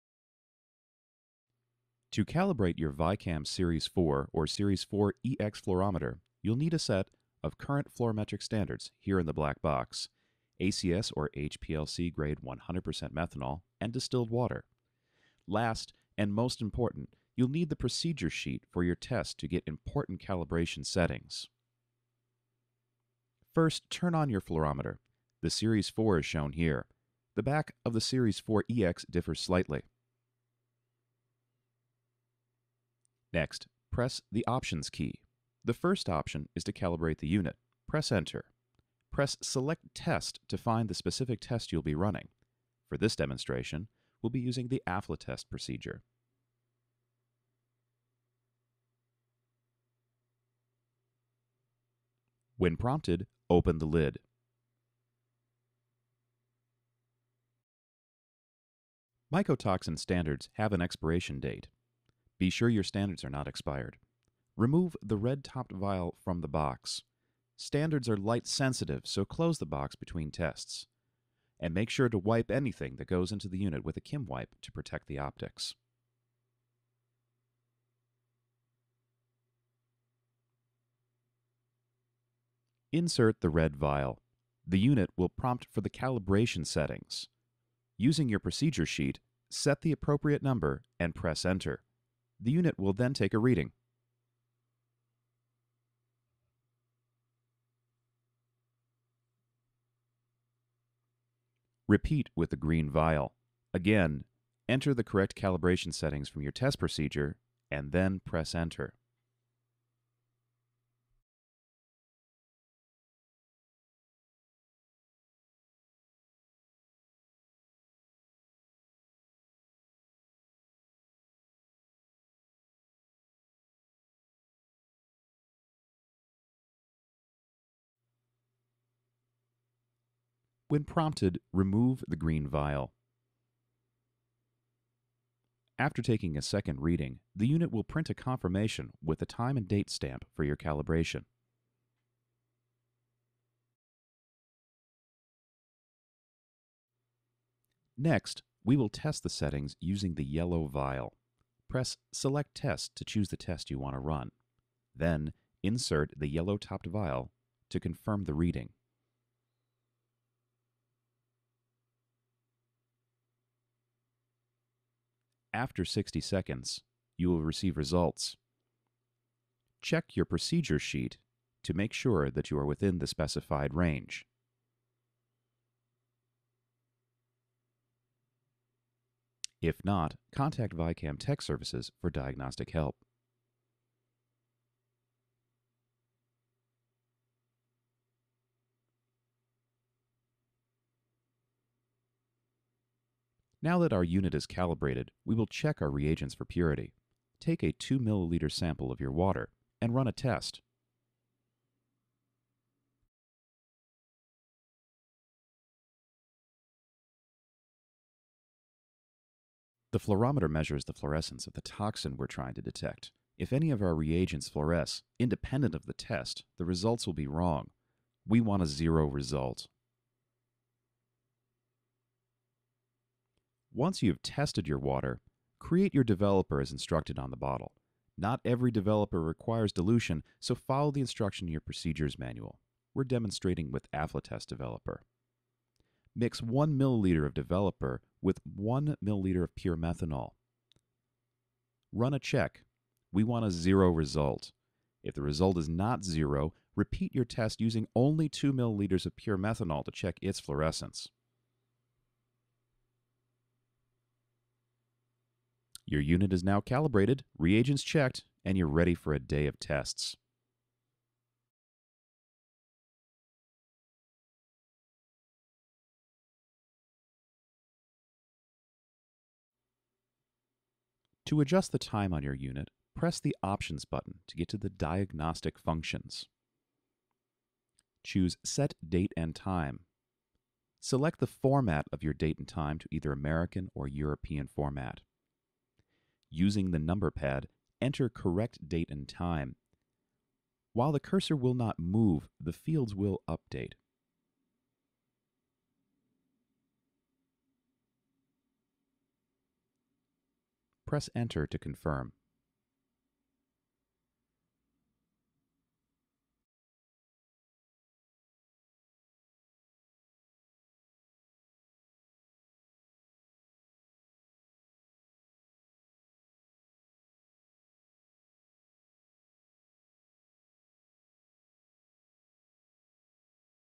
Fluorometer Calibration Instructions
Instructions for calibrating the VICAM Series 4 and Series 4EX fluorometer

VICAM Series 4EX Fluorometer Calibration